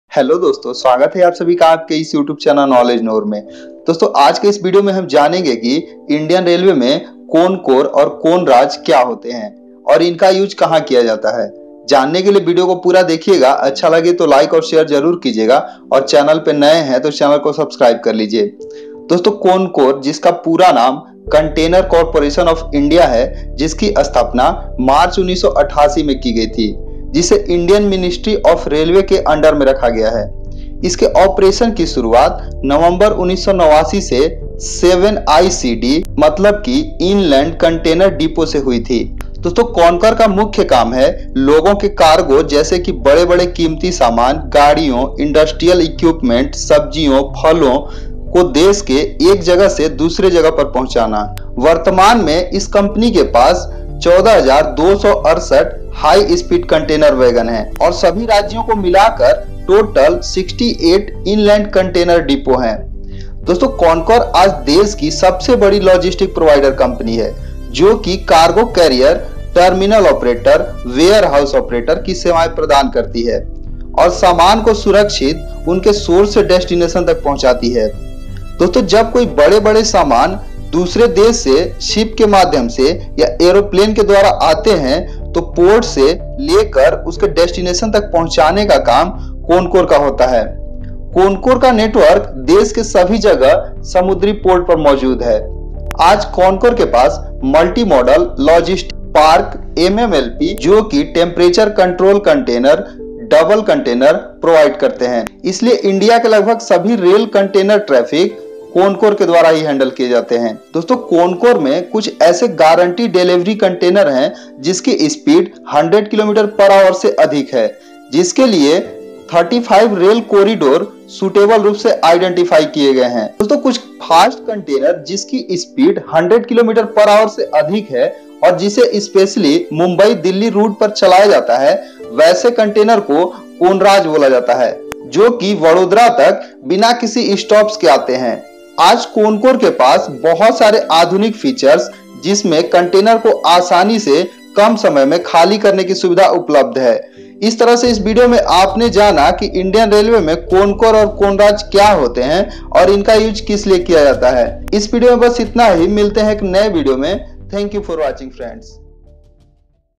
What is CONCOR & CONRAJ in indian railway?
Dosto es video me indian railway k CONCOR and CONRAJ ke bare me bataya gya hai...ye kya hote hai aur enka use kisliye kiya jata hai janane k liye video ko pura dekhiye achha lage to like kijiye share kijiye